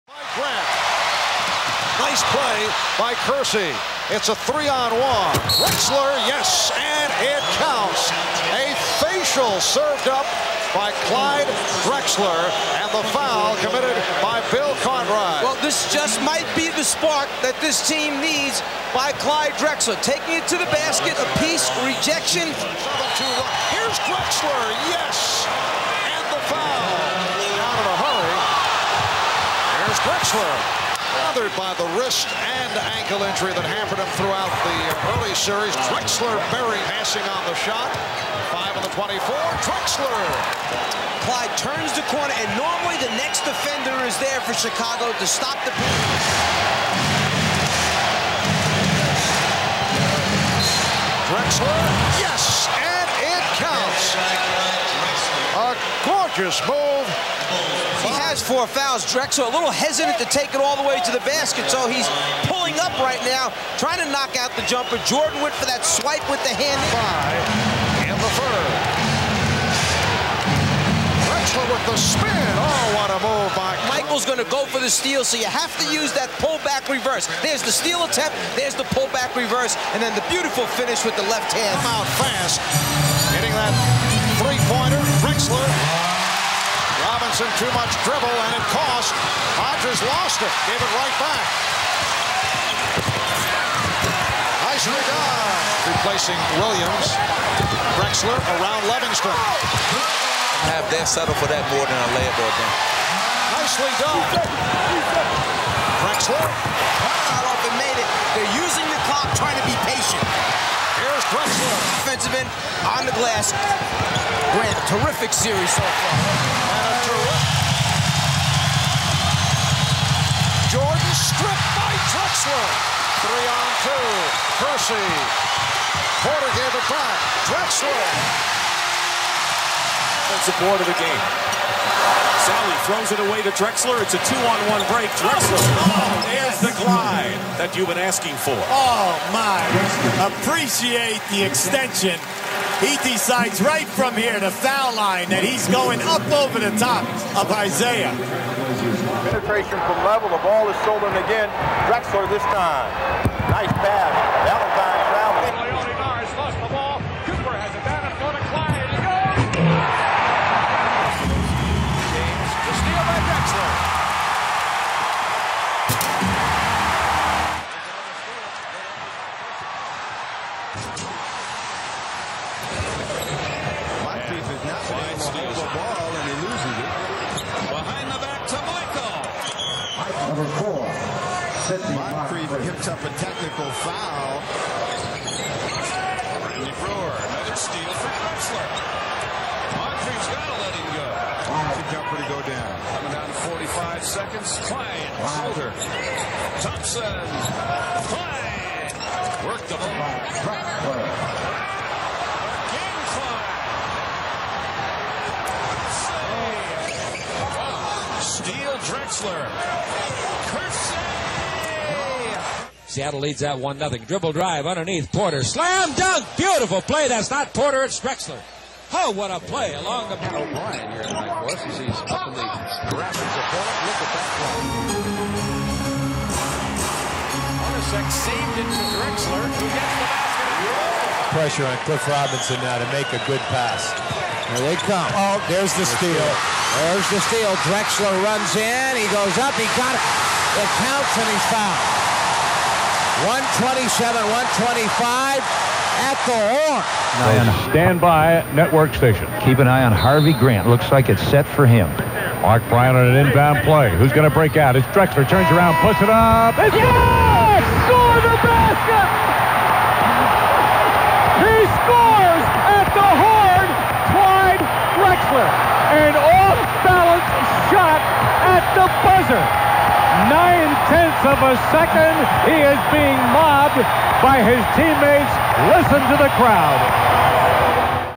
Clyde Drexler's Best Trail Blazers Highlights
The biggest plays of The Glide's career in Portland.